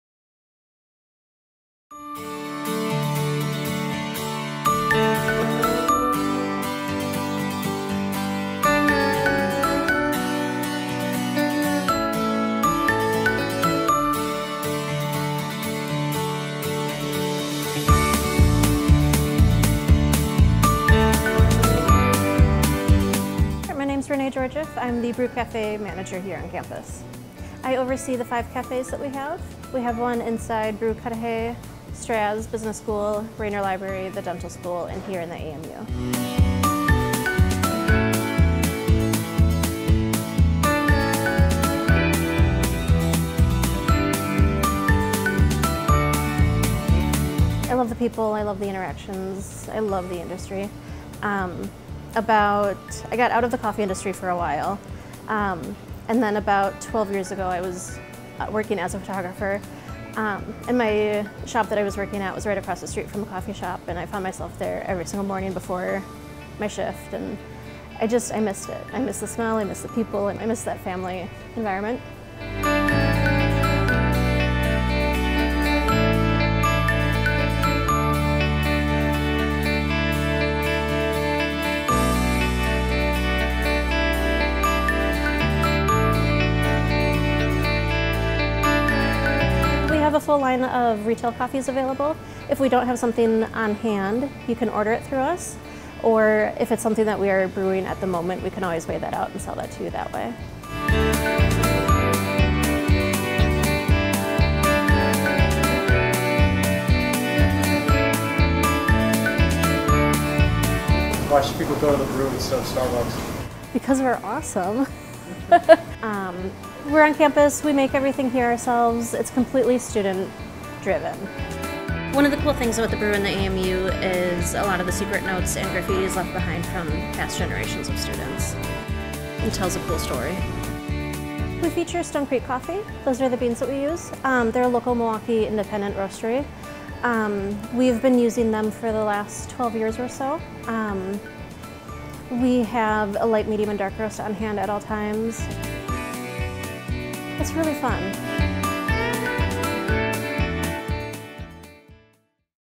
Meet the Manager - Renee Georgiev
Renee Georgiev, the Brew Cafe Manager at Marquette University, discusses her love for coffee and what The Brew provides to students and staff.